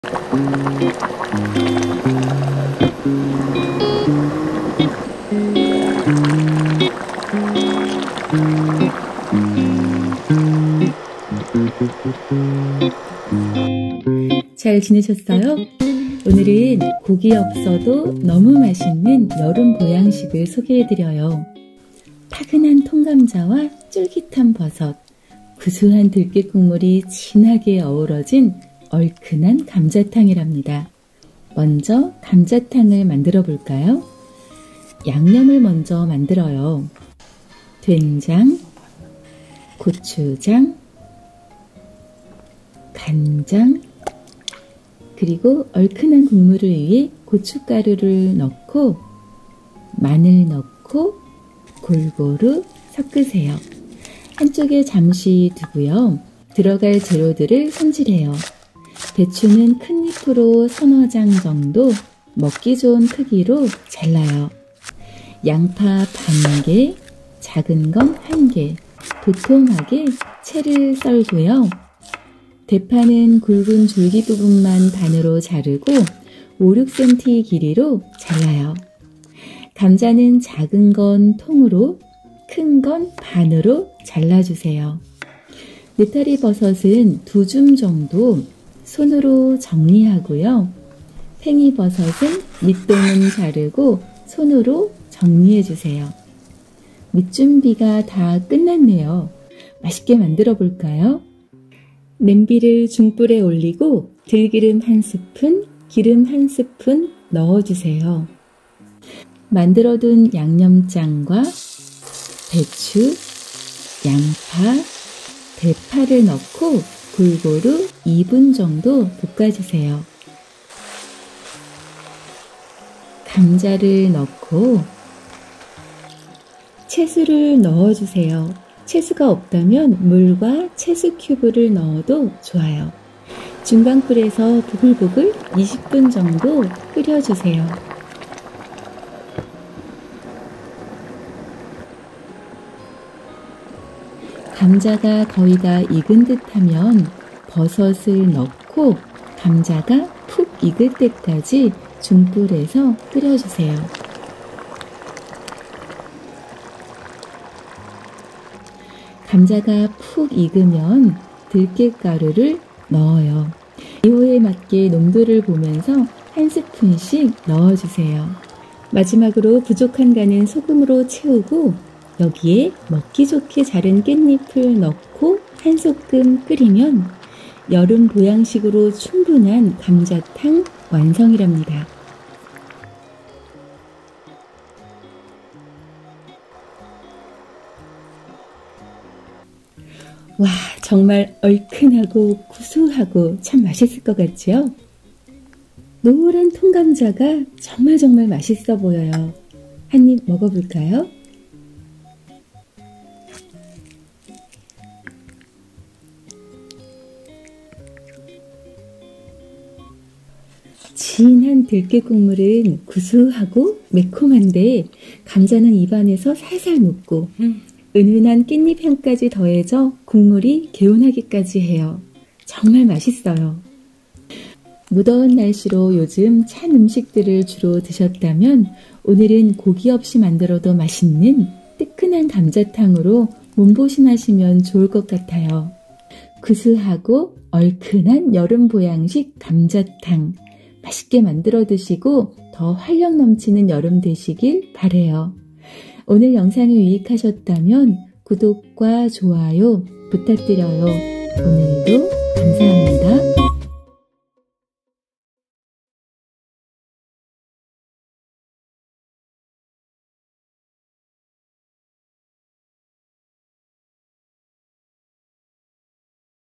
감자탕 고기 없이 만들기! 파근한 통감자와 들깨국물이 진하게 어우러진 감자탕! 너무 맛있답니다 :)
고기 없어도 너무~ 맛있는 여름 보양식. 파근한 통감자와 쫄깃한 버섯, 구수한 들깨국물이 진하게 어우러진 얼큰한 감자탕이랍니다. 여름을 지혜롭게 나는 방법, 이열치열! 매콤한 감자탕으로 시원한 여름되세요!

🌿Plant-based | Healthy Food Cuisine
🙋🏻‍♀️Claire정아